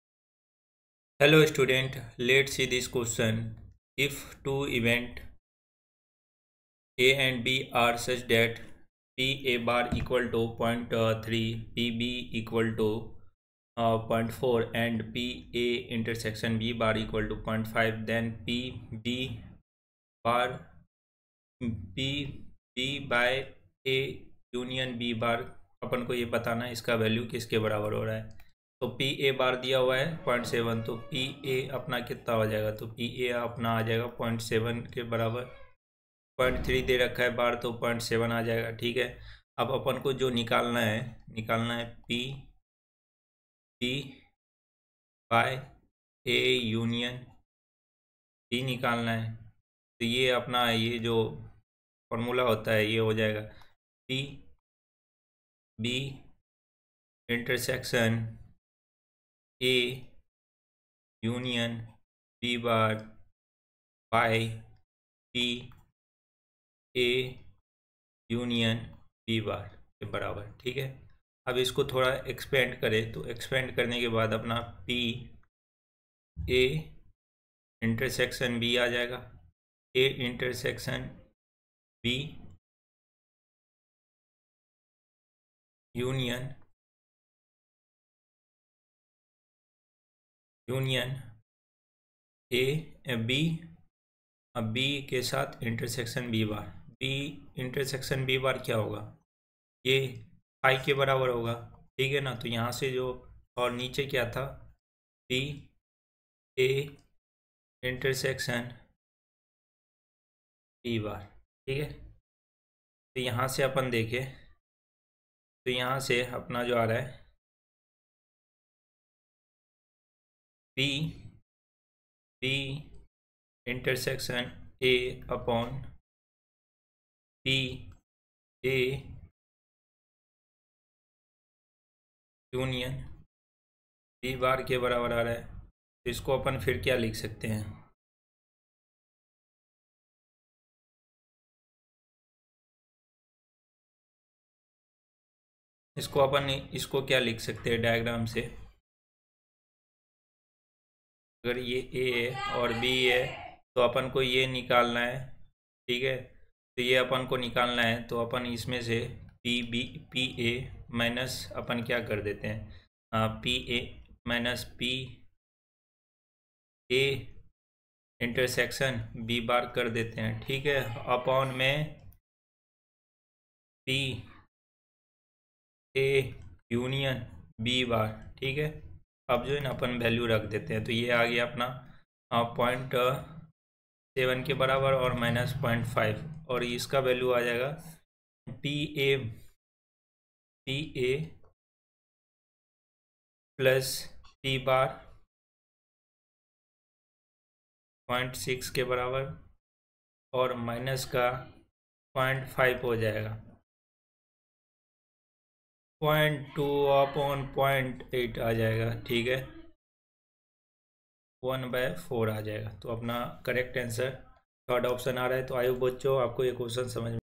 If two events \( \mathrm{A} \) and \( \mathrm{B} \) are such that \( P(\bar{A})=0.3 \), \( P(B)=....
If two events \( \mathrm{A} \) and \( \mathrm{B} \) are such that \( P(\bar{A})=0.3 \),
\( P(B)=0.4 \) and \( P(A&cap;B)=0.5 \) then \( P(B \mid(A \cup \bar{B})) \) equals- 📲PW App